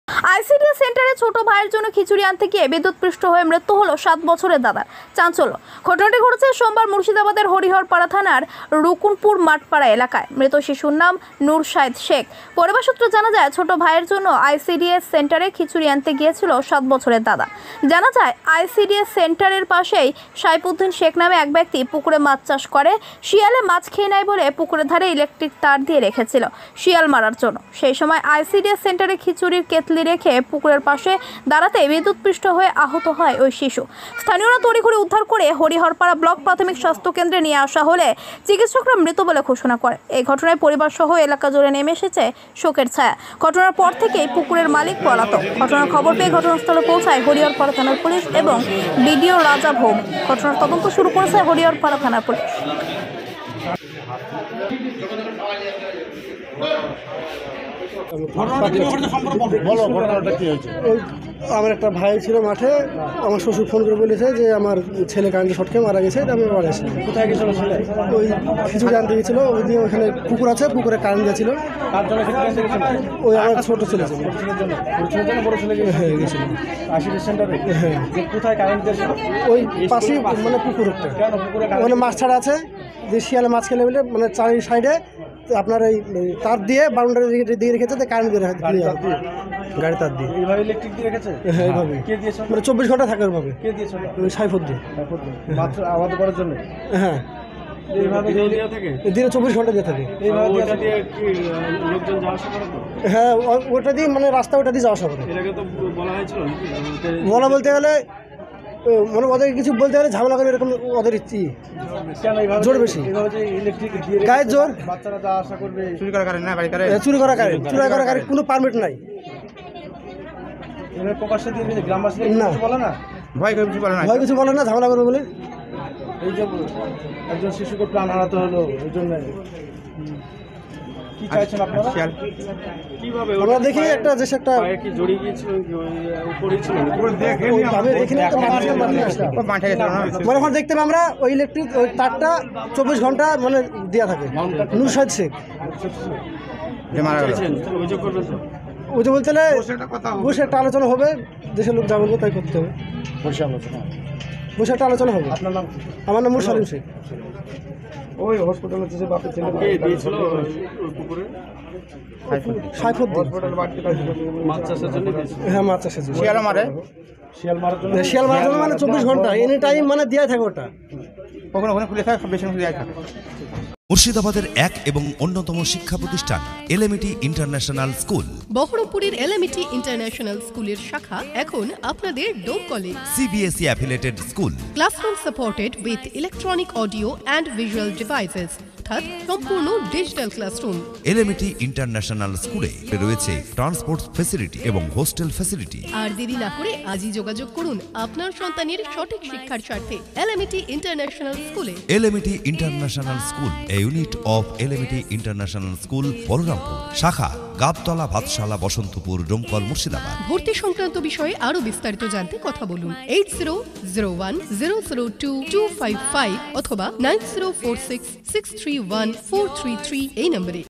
আইসিডিএস সেন্টারে ছোট ভাইয়ের জন্য খিচুড়ি আনতে গিয়ে বিদ্যুৎপৃষ্ট হয়ে মৃত্যু হল সাত বছরের দাদা
আইসিডিএস সেন্টারে ছোট ভাইয়ের জন্য খিচুড়ি আনতে গিয়ে বিদ্যুৎপৃষ্ট হয়ে মৃত্যু হল সাত বছরের দাদার চাঞ্চল্য
ঘটনাটি ঘটেছে সোমবার মুর্শিদাবাদের হরিহরপাড়া থানার রুকুনপুর মাঠপাড়া এলাকায়।